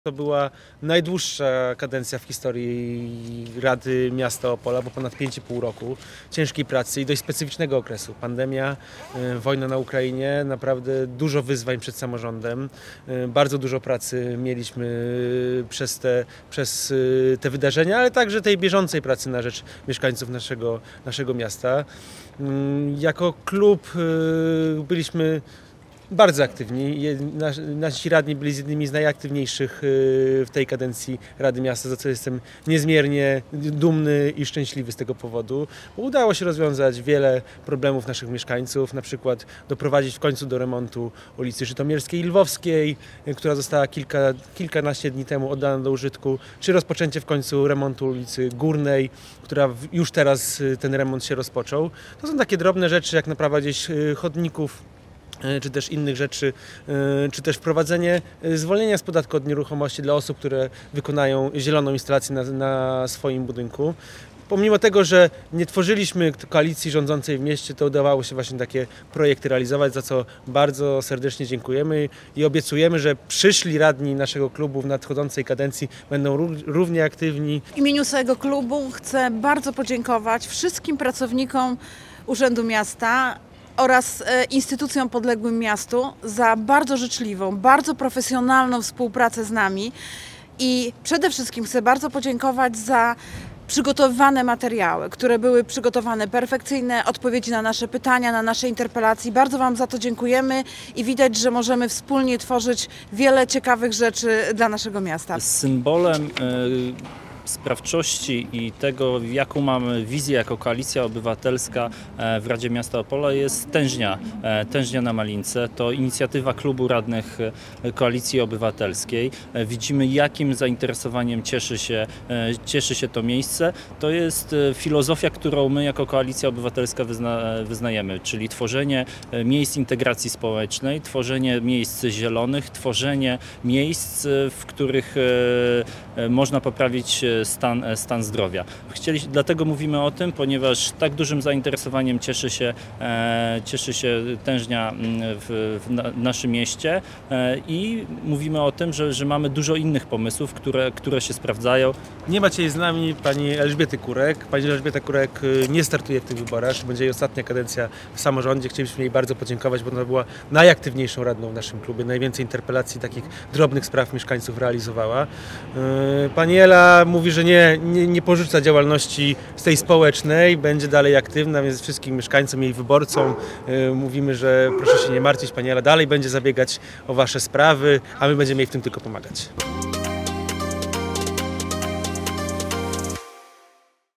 PO podsumowuje kadencję w Radzie Miasta Opola
To była najdłuższa kadencja i mimo, że nie byliśmy w miejskiej koalicji to wiele naszych projektów udało się zrealizować, a współpraca w radzie układała się zwłaszcza pod względem organizacyjnym wzorcowo - przyznawali radni miejscy Platformy Obywatelskiej.